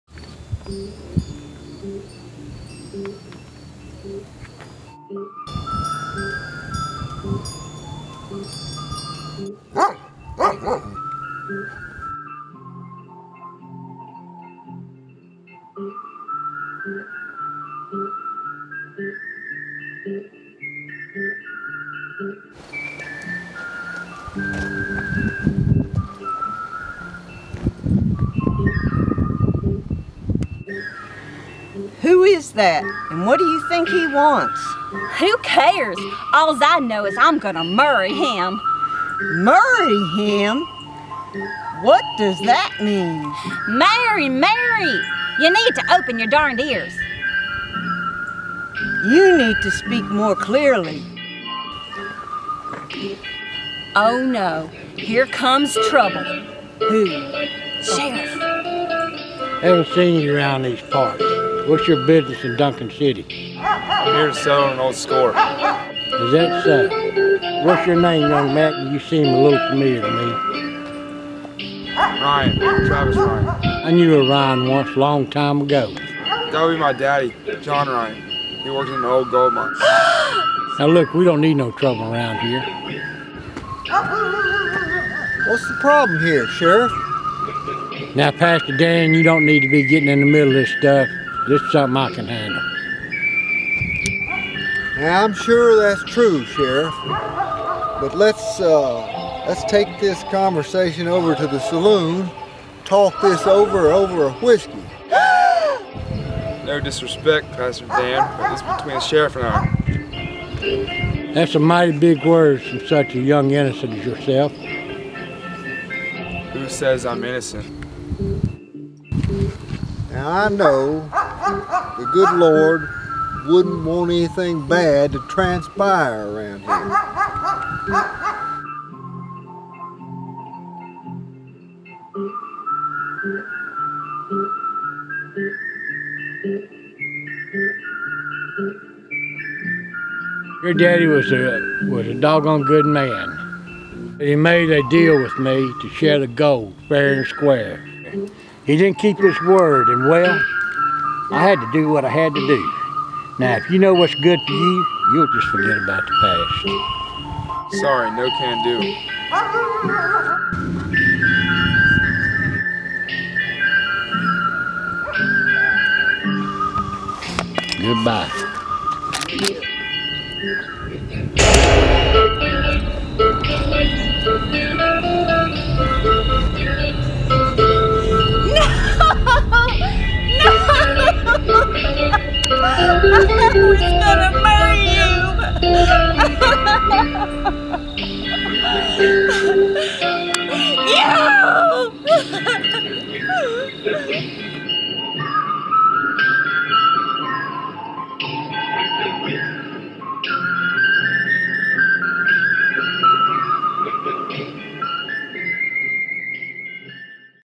Dunkin City
A young, handsome stranger comes to Dunkin City to seek revenge.

Starring Spencer Welch, Recon Bill, & Chris Welch. Co-Starring Cheyenne Townsend & Kelly Mullis. Written, Directed, Shot, Edited by Kelly Mullis on location in North Carolina.